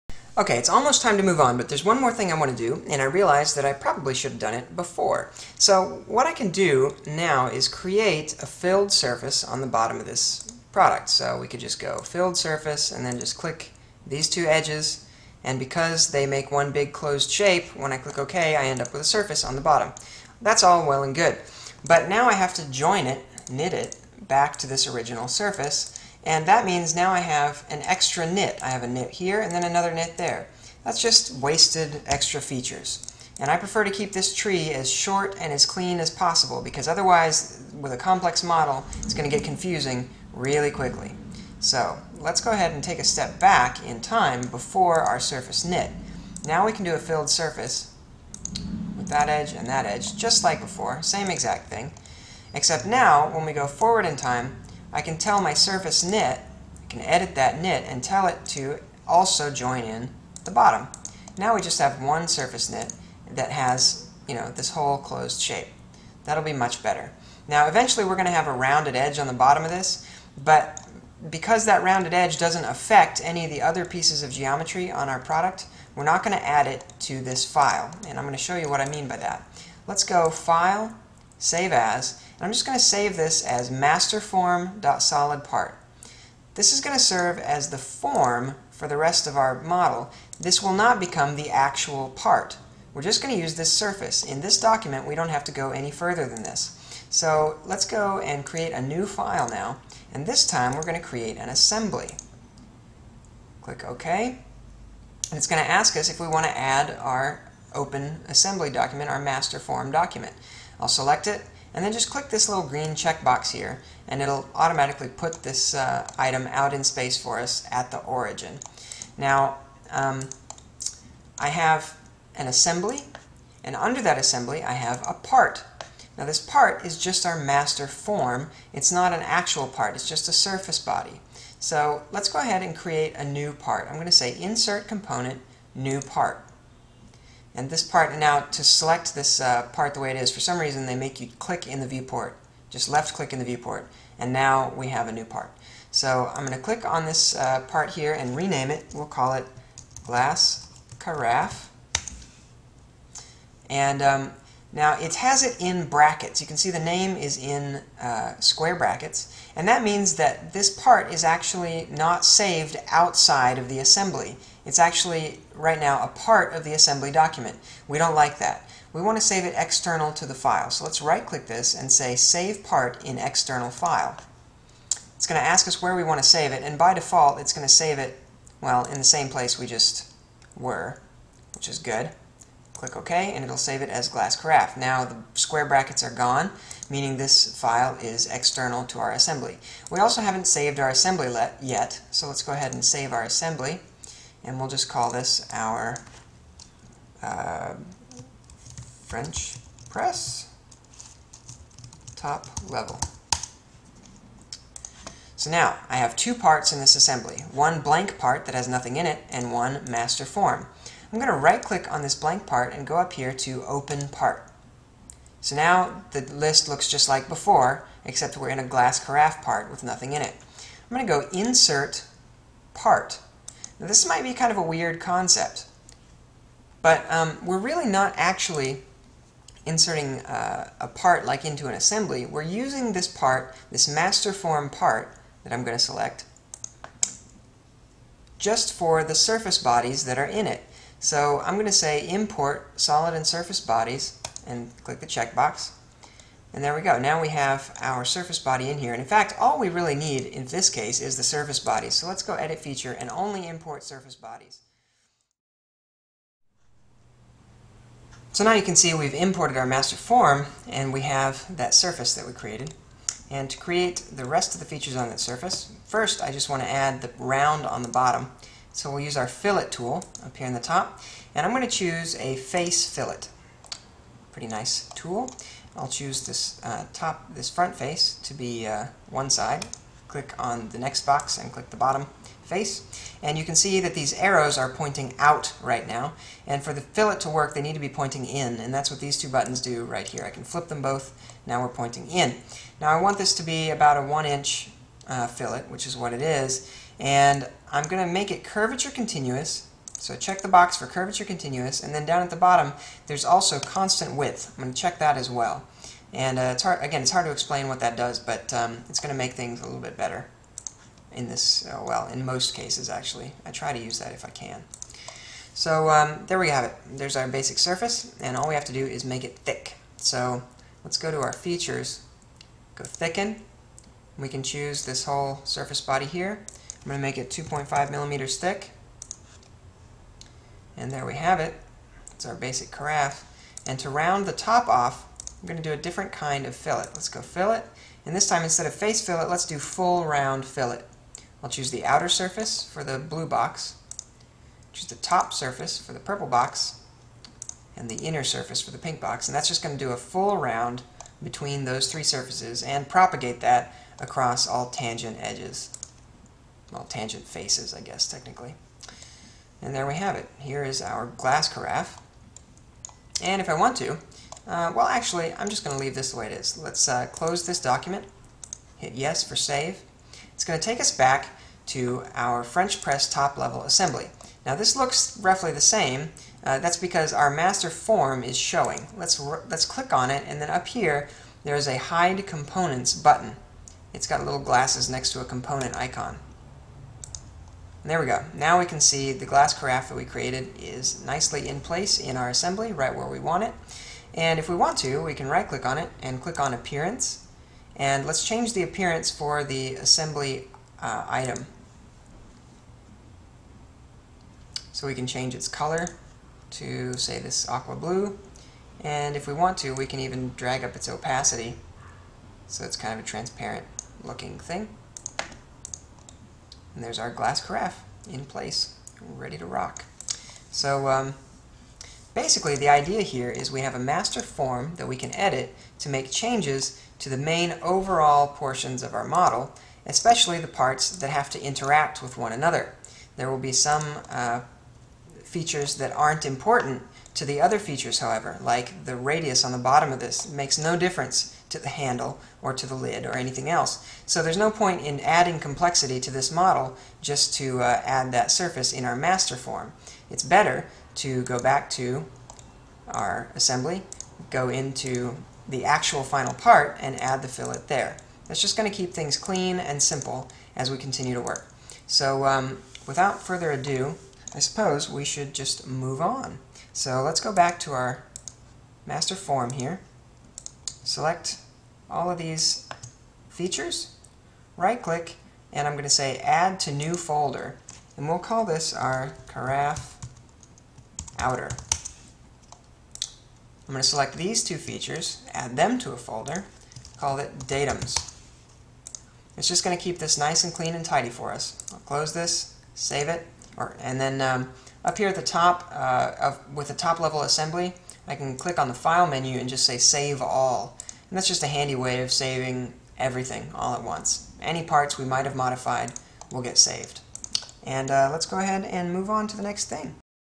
SolidWorks Class 101: coffee maker - video 04 of 16
Build an entire French Press coffee maker from scratch using SolidWorks 2010 in this ground-up tutorial. Start off with interface basics, followed by an introduction to the industrial design 'master model' workflow to create simple, easy to edit assemblies. Add embossed graphics from Illustrator, create assembly constraints, do a basic drawing, and even export to modo for rendering! Check it out in detail at CADjunkie.com!